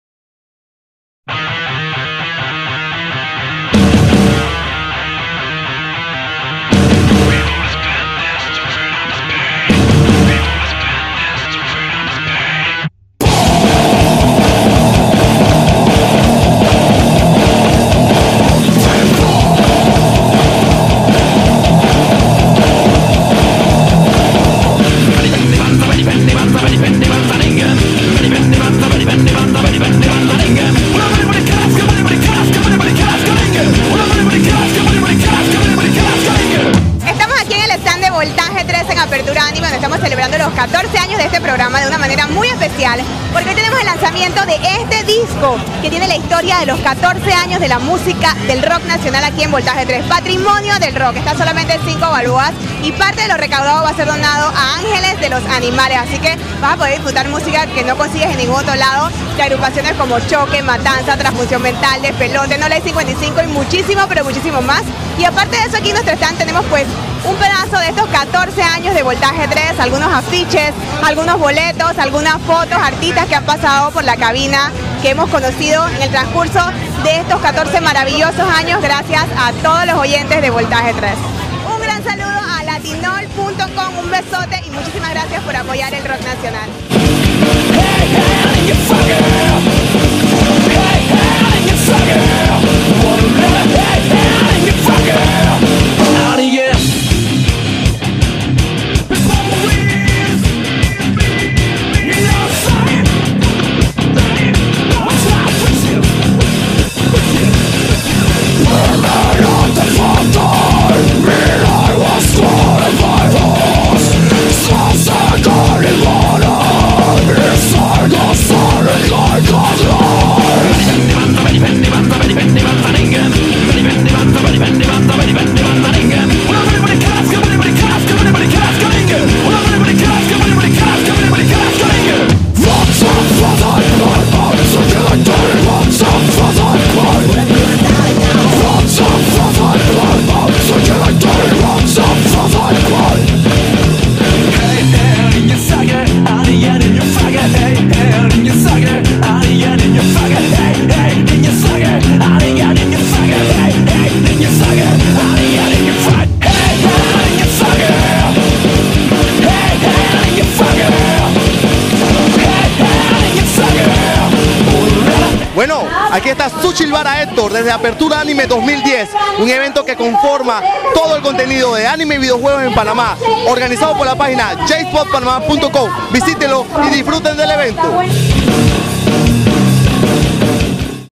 Apertura Anime 2010 por LatinOL.com
Aquí podrás ver todo lo que sucedió en Apertura Anime 2010 y cada uno de los Cosplay que participaron en la competencia.